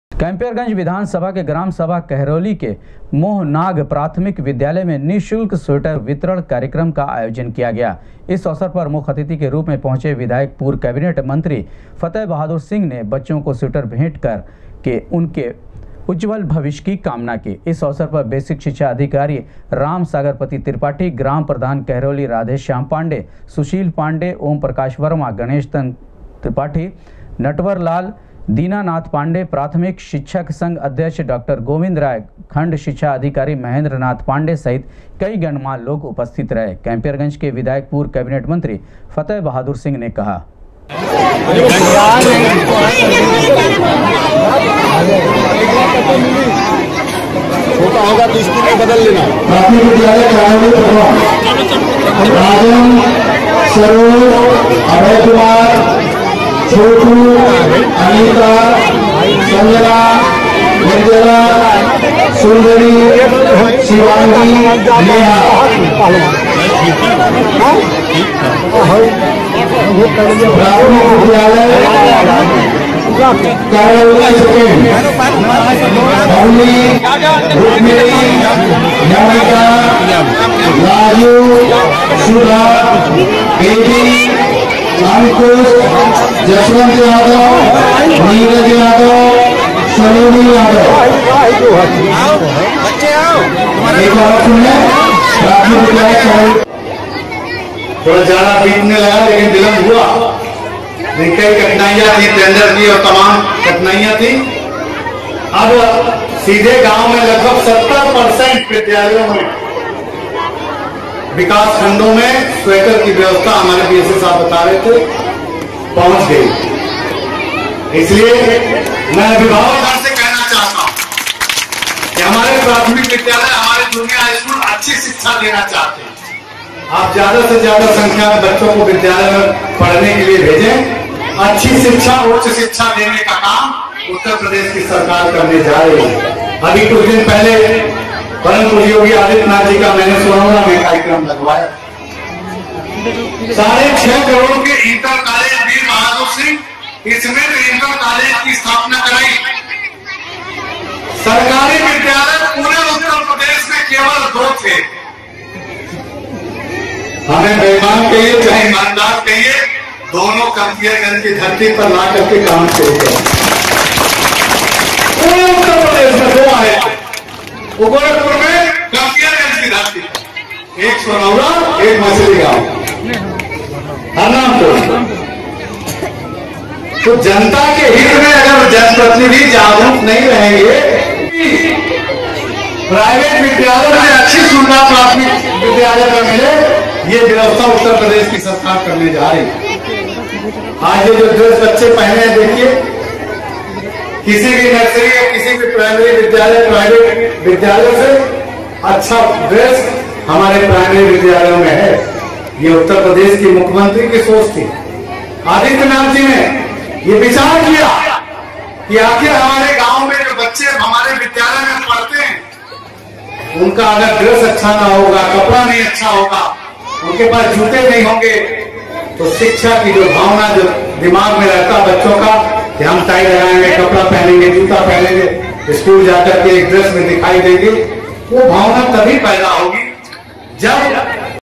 विधायक एवं पूर्व कैबिनेट मंत्री फतेह बहादुर सिंह ने बच्चों को भेट किया निःशुल्क स्वेटर
विधायक एवं पूर्व कैबिनेट मंत्री फतेह बहादुर सिंह ने बच्चों को भेट किया निःशुल्क स्वेटर  GORAKHPUR NEWS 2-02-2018